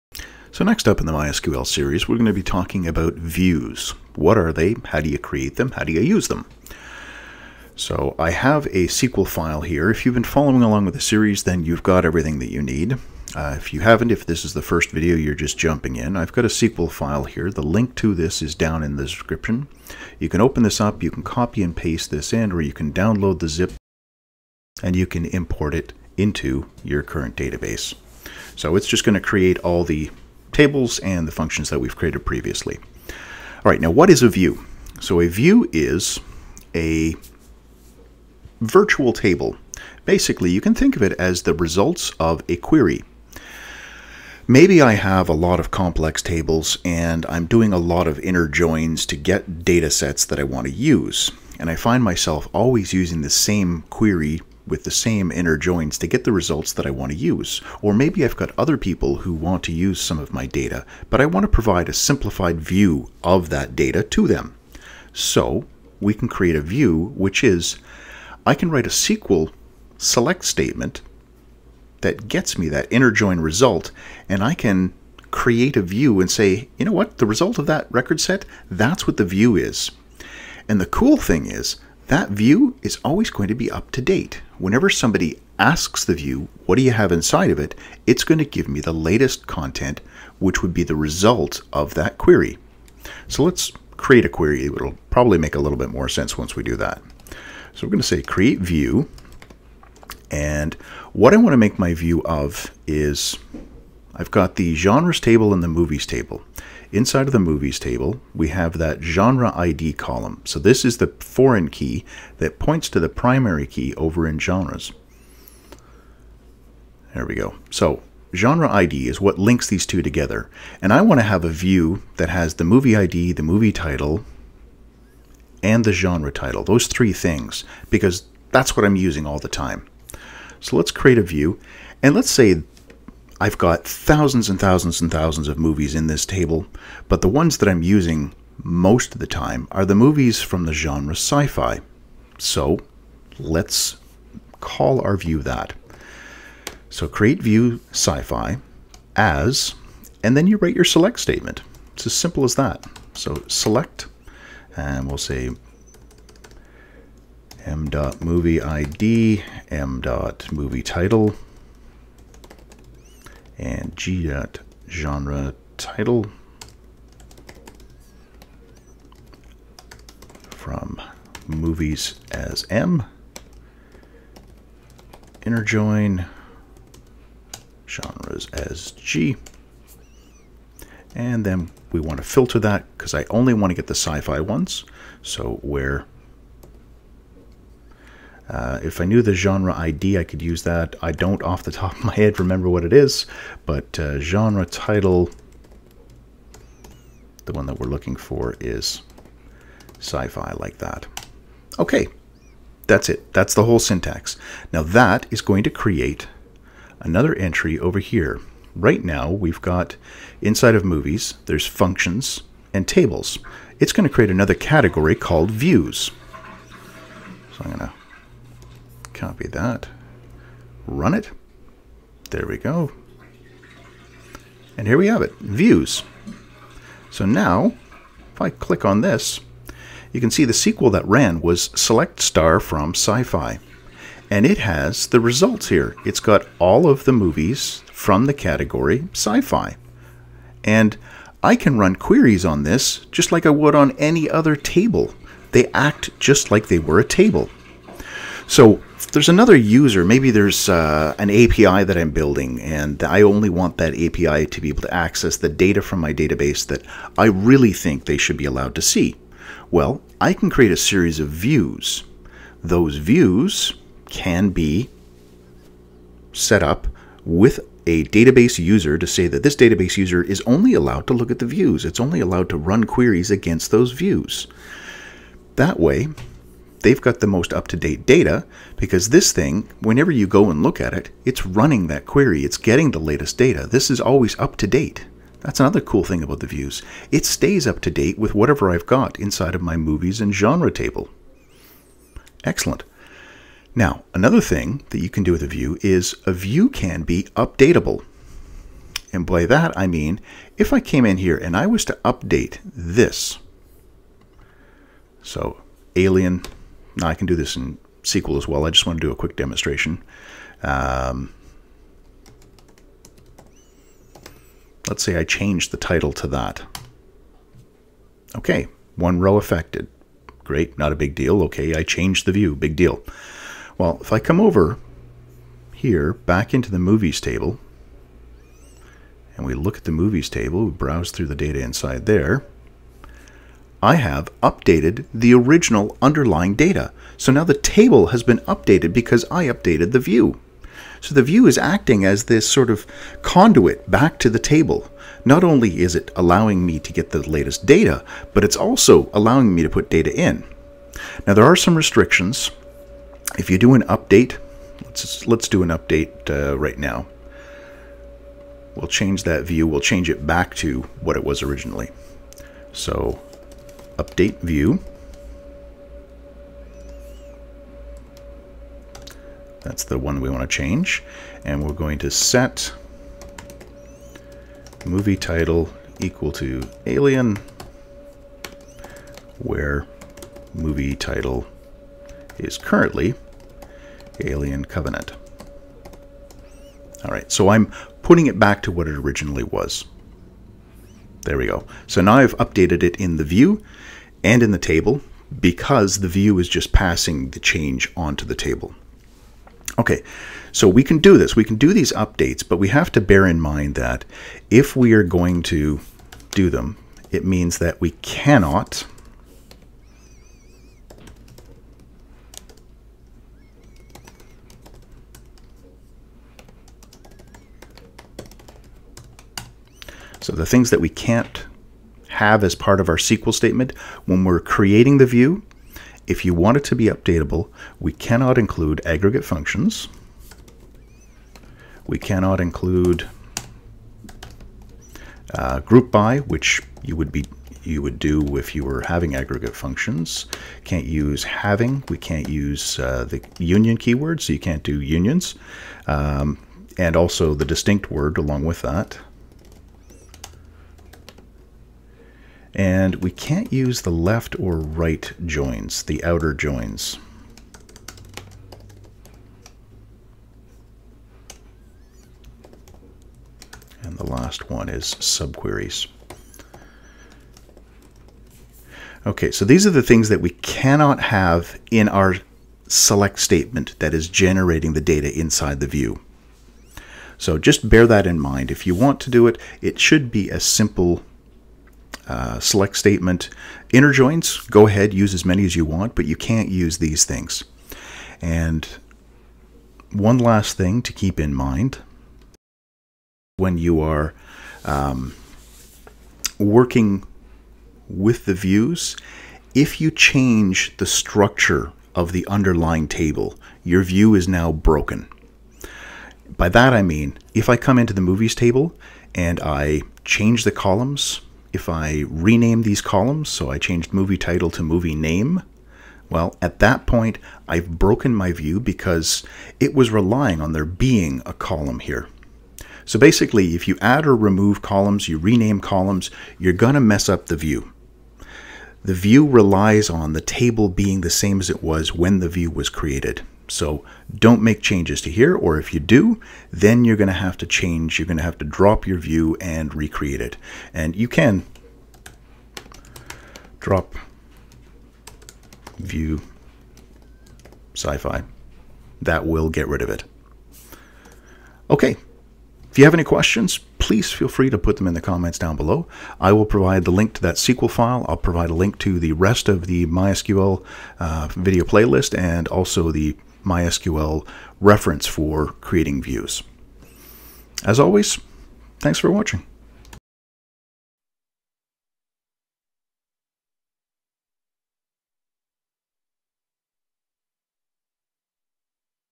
Learning MySQL - CREATE VIEW
This episode covers how to create and work with views. Be sure to watch to the end to get all the gotchas.